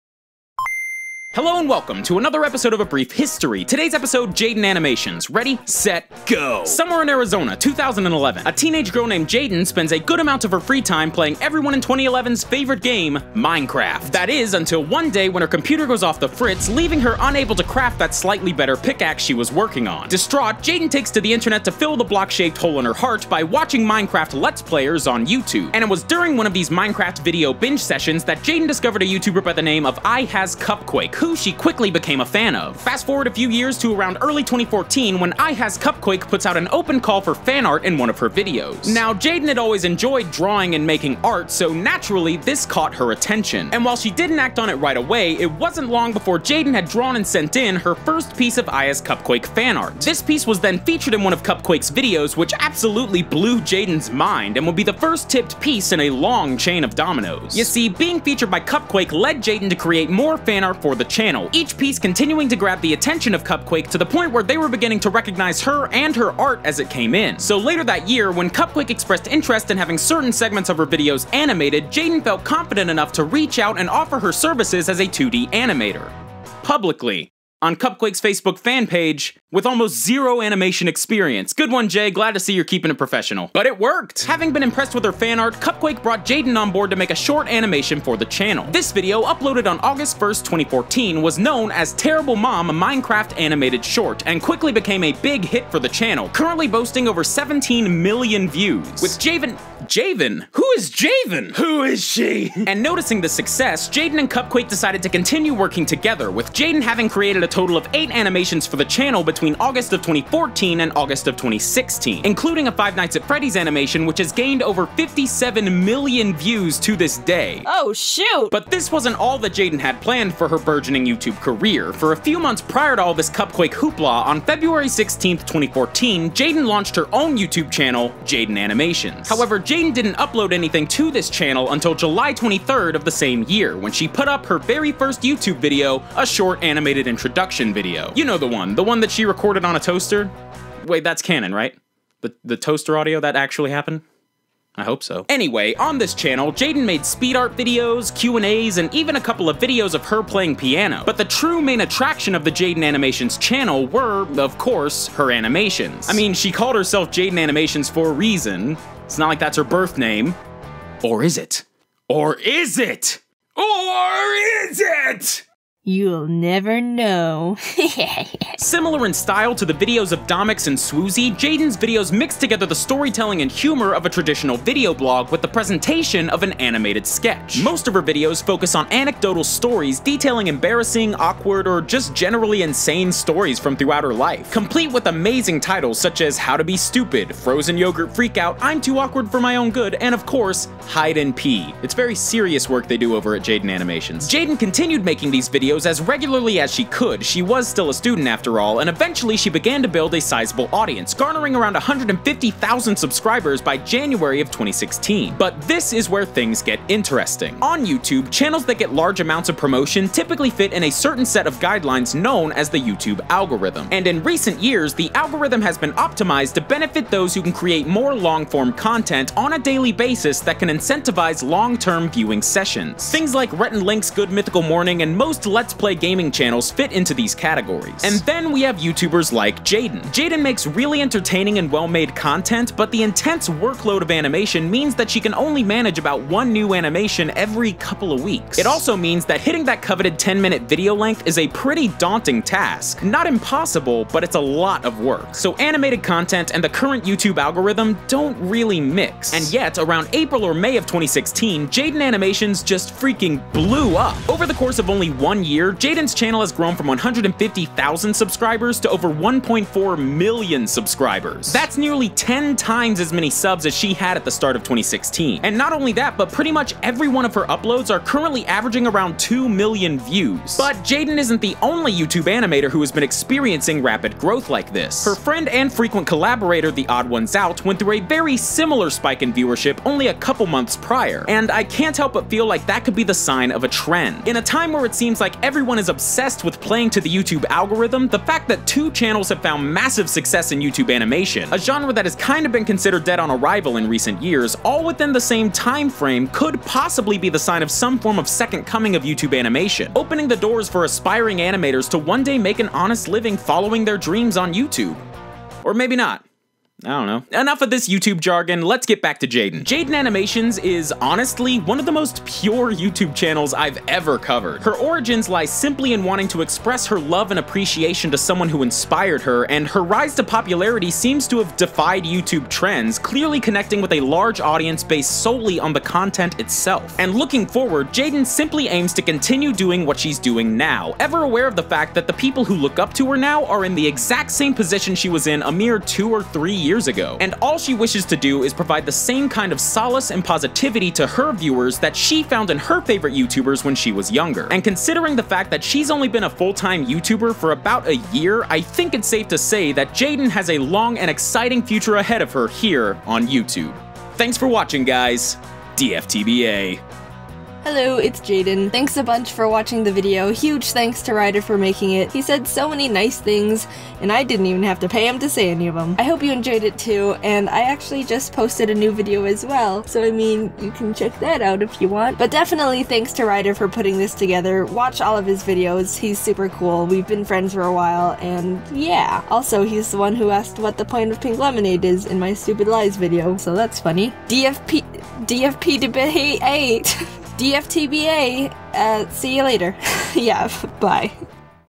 The History Of JaidenAnimations (Feat. Jaiden) | A Brief History
One of 2016's biggest YouTube MVP's came in the form of an animated storyteller named JaidenAnimations. Having jumped from 150,000 subs to over 1.4 million in just a year, Jaiden has clearly struck a chord with the YouTube community. And today we take a look back to see how she got started!

Massive thank you to Jaiden for helping me out with this video!